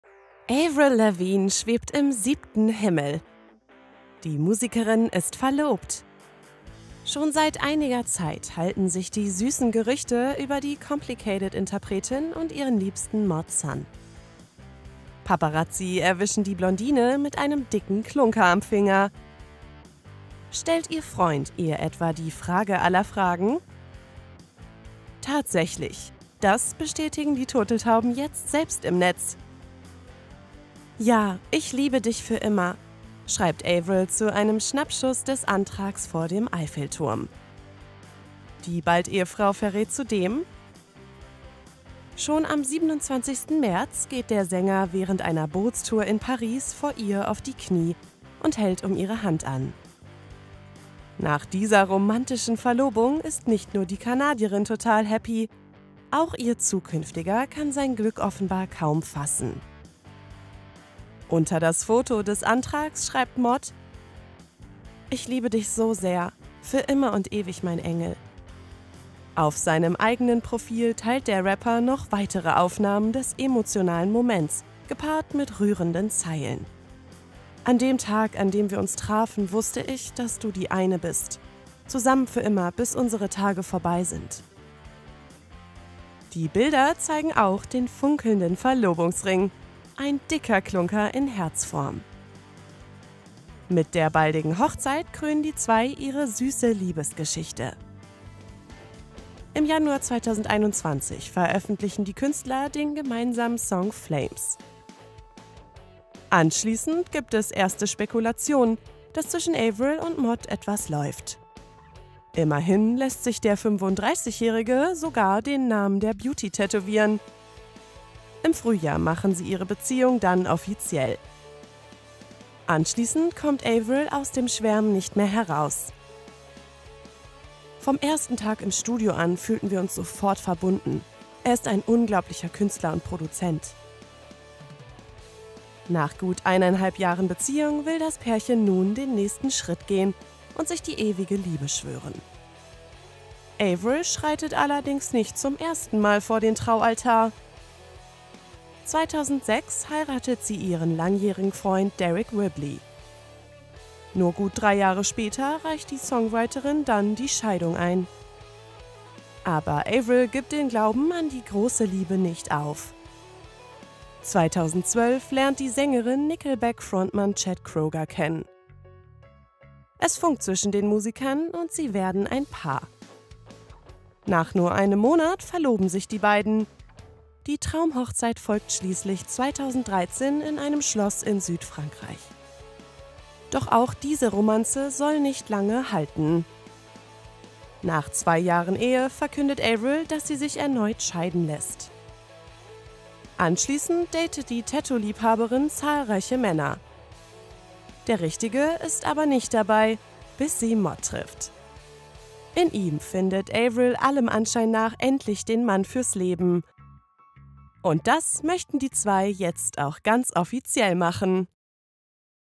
Romantisch in Paris! Avril Lavigne und Mod Sun sind verlobt
Süße News von Avril Lavigne (37) – die Musikerin wird heiraten! Seit gut eineinhalb Jahren datet die Sängerin ihren Kollegen Mod Sun (35). Erst kürzlich kamen dann Gerüchte auf, dass sich die Blondine und der Rapper verlobt hätten. Denn die Kanadierin wurde mit einem dicken Klunker am Finger abgelichtet. Jetzt bestätigte die 37-Jährige die Spekulationen: Ihr Liebster hat in Paris tatsächlich um ihre Hand angehalten! "Ja, ich liebe dich für immer!", schwärmte Avril auf Instagram. (prh/sre)